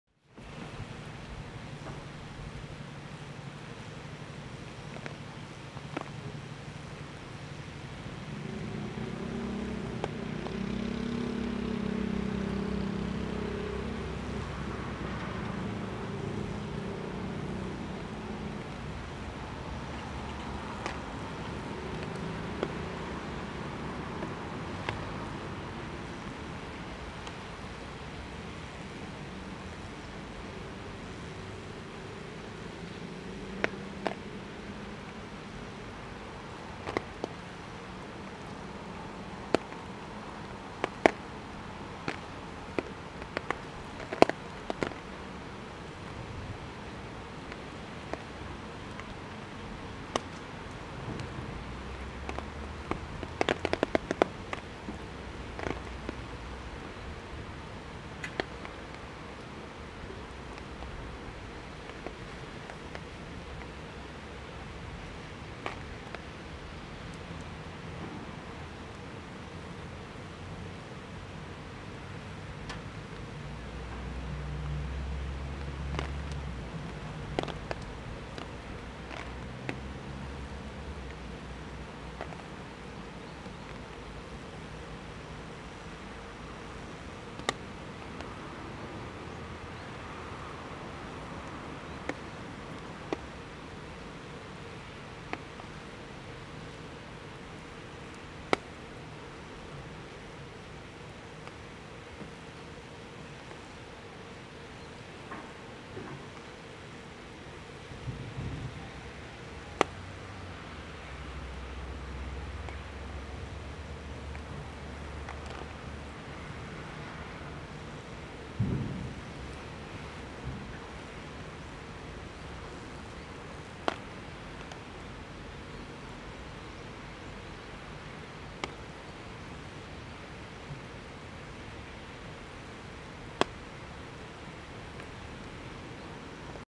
マーガレットと雨の雫 / Marguerite and rain drops
雨のしずくをまとった黄色いお花。

近くで見るとしずくってとってもきれい♪

大変な時期ですが、この動画を見て少しでもリラックスに繋がったらいいなと思っています。